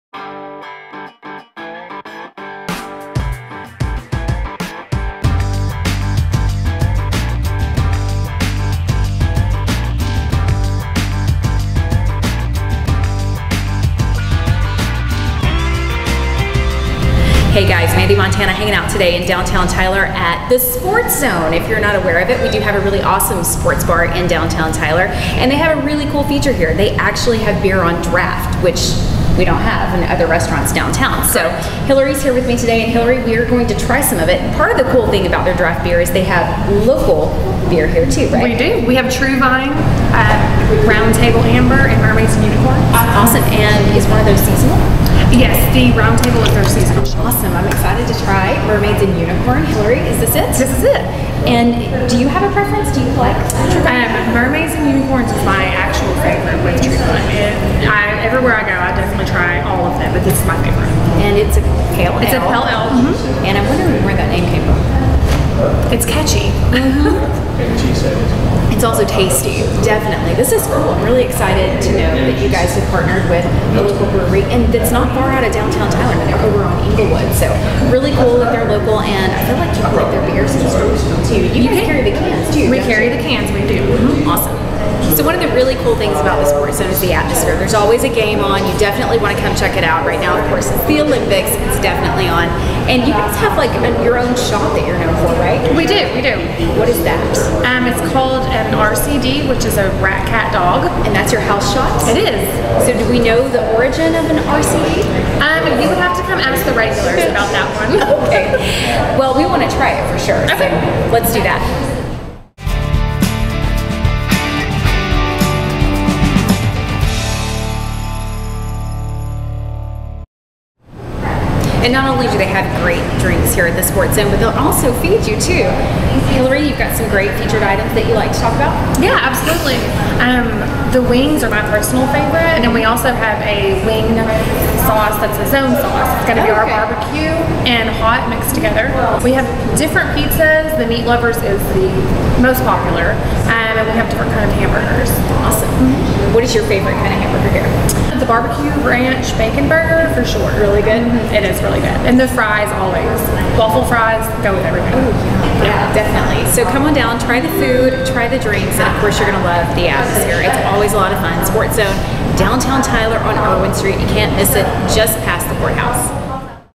Mandee About Town - Sports Zone
Mandee continues her trek through downtown Tyler, hanging out at all the best spots. Today she checks out The Sports Zone, downtown Tyler's only bar with draft beer.